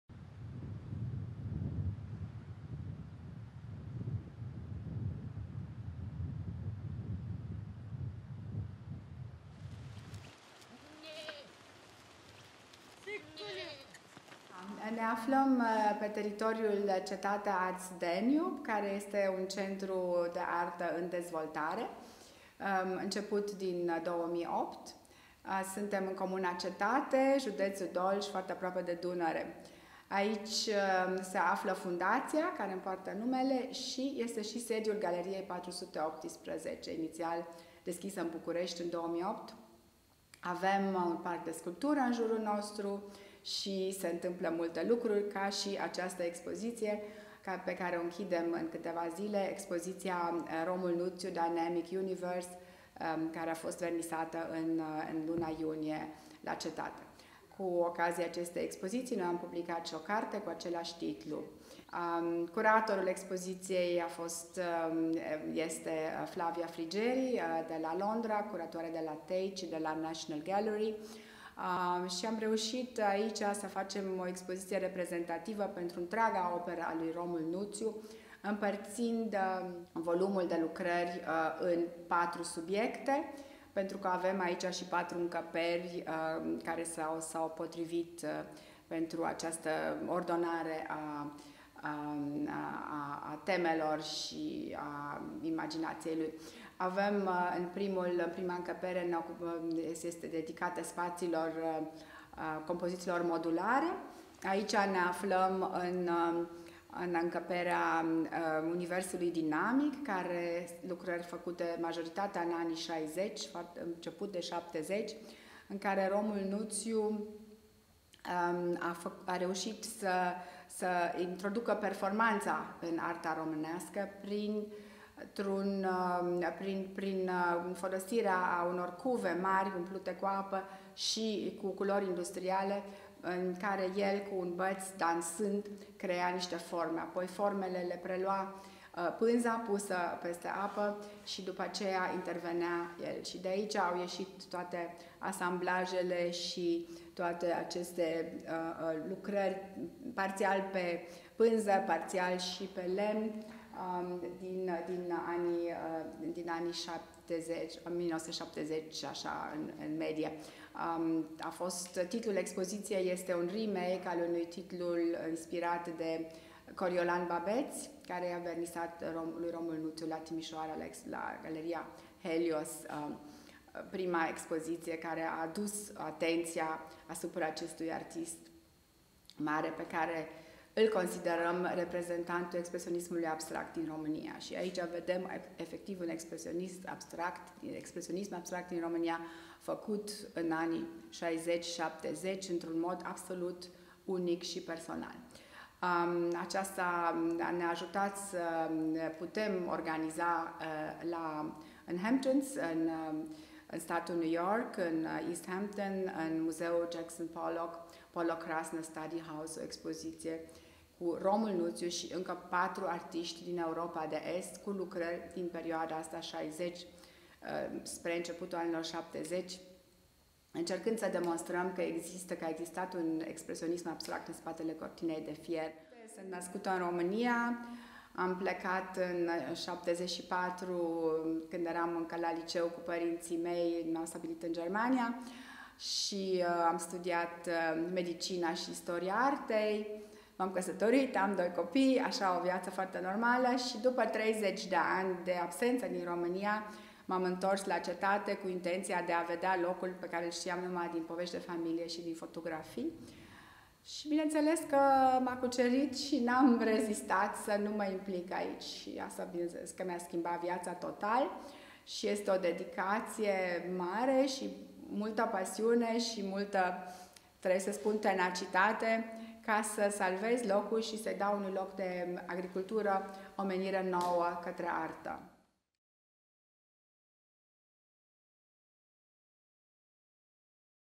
With Romul Nutiu in Cetate- Cu Romul Nutiu la Cetate
Exhibition: Dynamic Universe, curator Dr. Flavia Frigeri
Date: June 27th - October 27th, 2022
Location: 418GALLERY - Cetate Arts Danube
Video: Petrut Calinescu